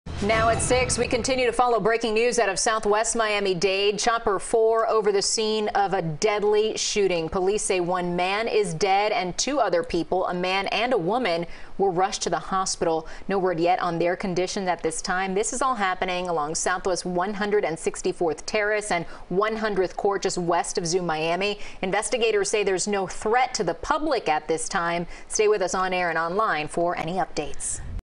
1 dead, 2 transported to hospital in SW Miami-Dade shooting
One person is dead, two others were transported to the hospital following a shooting in SW Miami-Dade Friday afternoon.